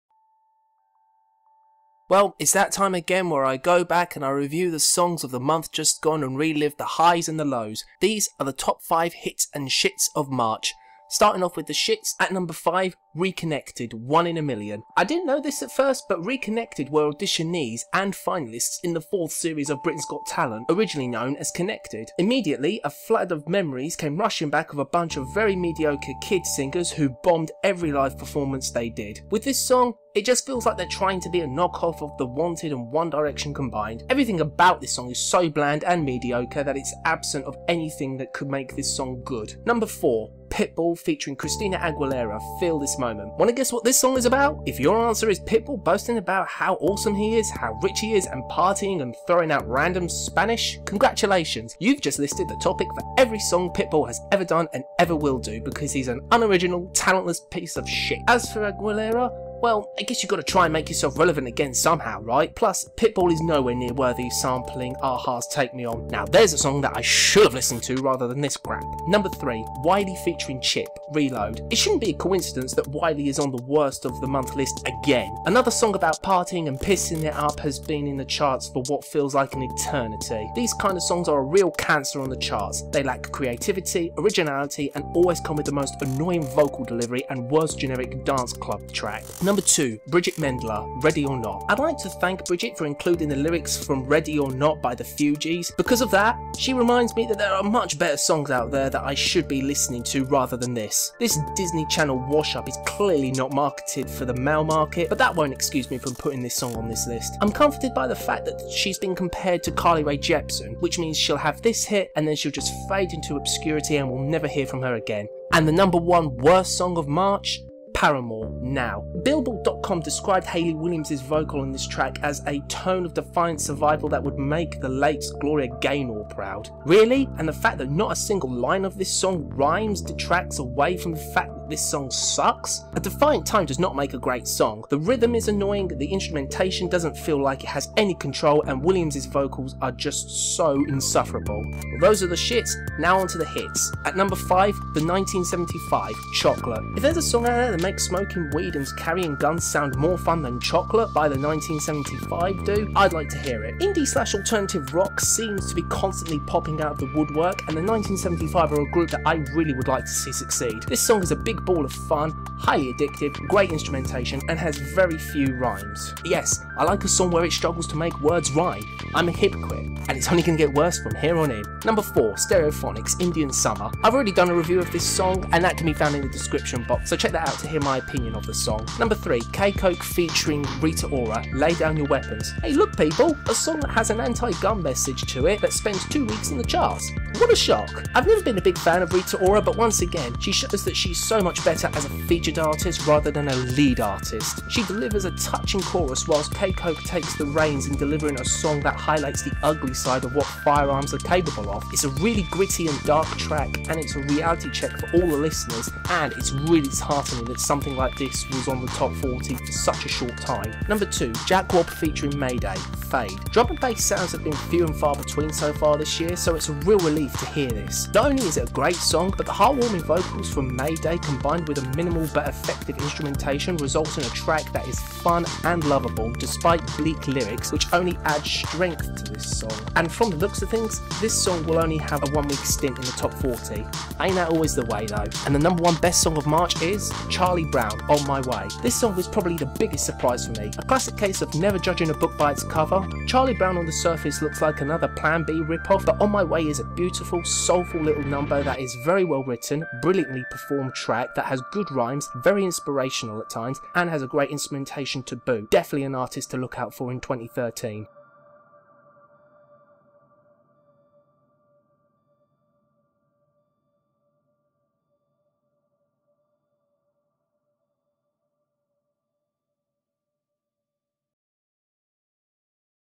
Top 5 Hits and Sh!ts of March
Another installment of Hits and Sh!ts where I remind you of the best and worst of the month just gone.